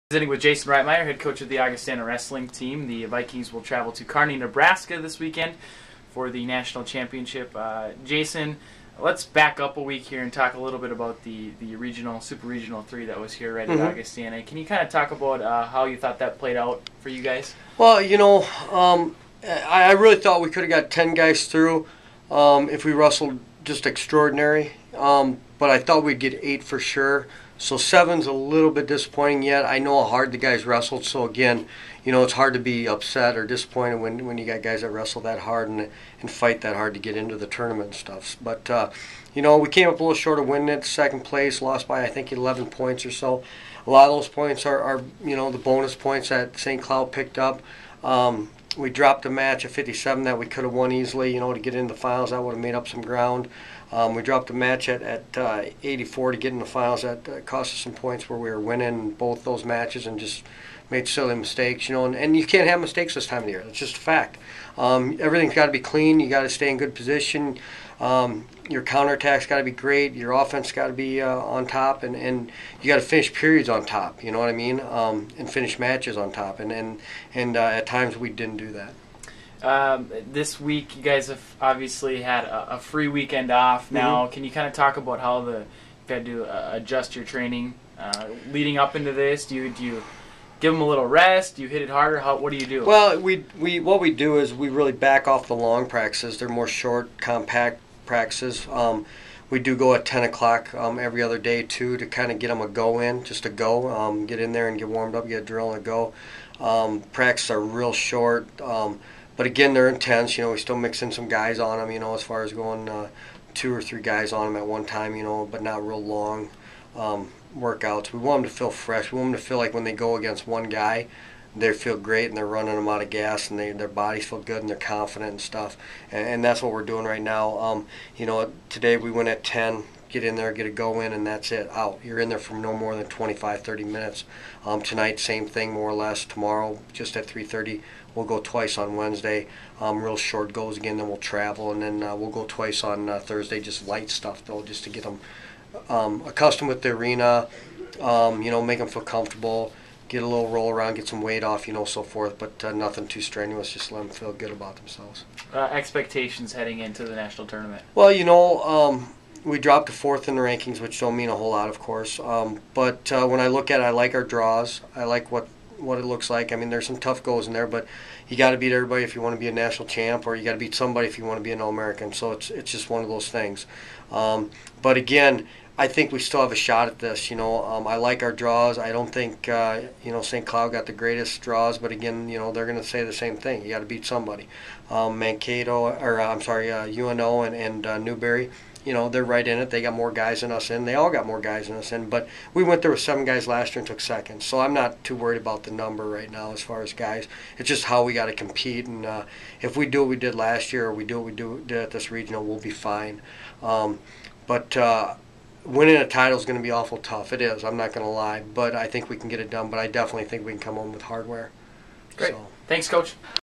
Wrestling-Jason Reitmeier NCAA Championship Preview
Head Coach Jason Reitmeier talks about his expectations at the upcoming NCAA Wrestling Championship.